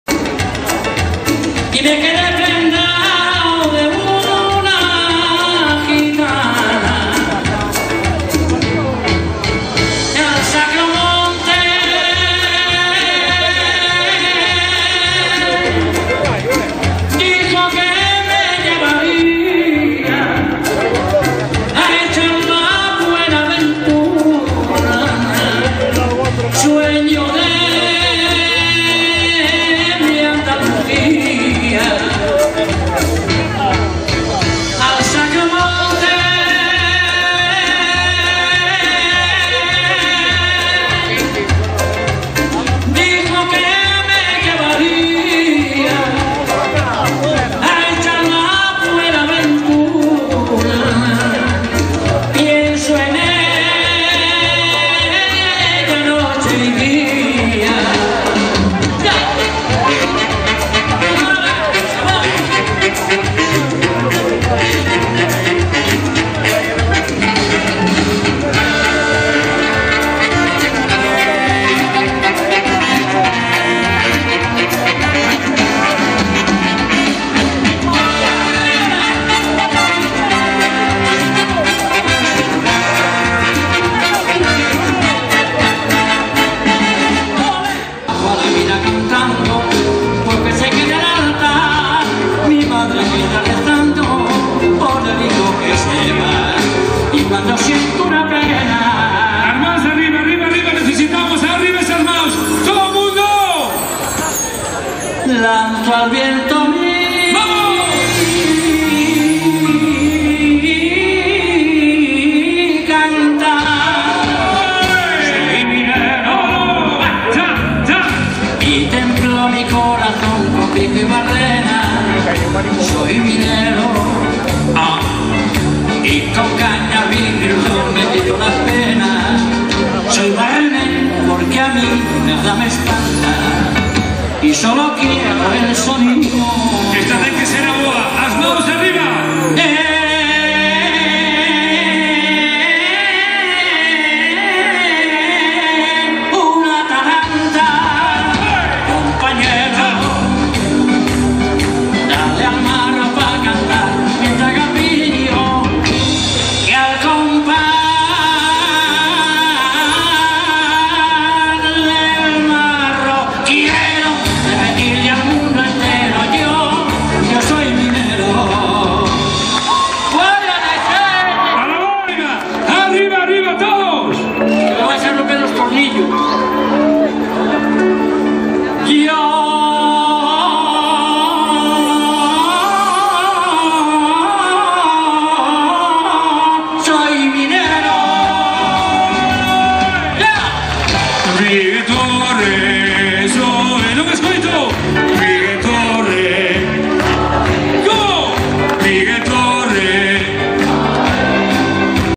poceiro en san antonio2014
En el día de inicio de las fiestas del monfortino barrio de San Antonio, actuó la orquesta Poceiro.